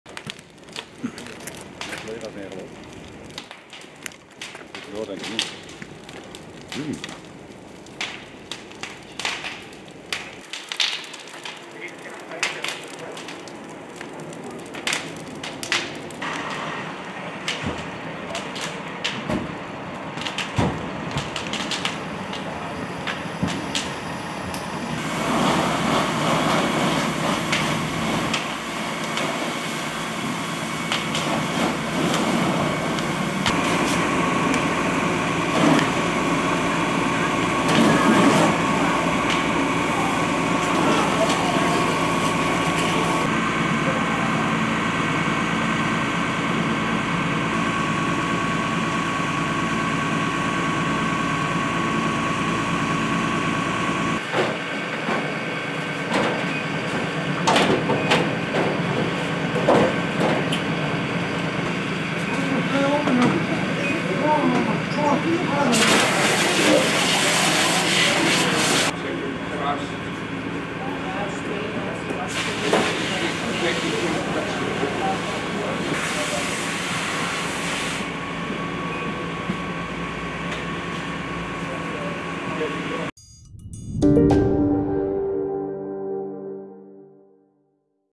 Auto gaat in vlammen op in Hengelo
In Hengelo is rond vier uur vannacht een auto in vlammen opgegaan. In de Paul Krugerstraat stond de auto geparkeerd onder een onderdoorgang van woningen, toen het voertuig vlam vatte.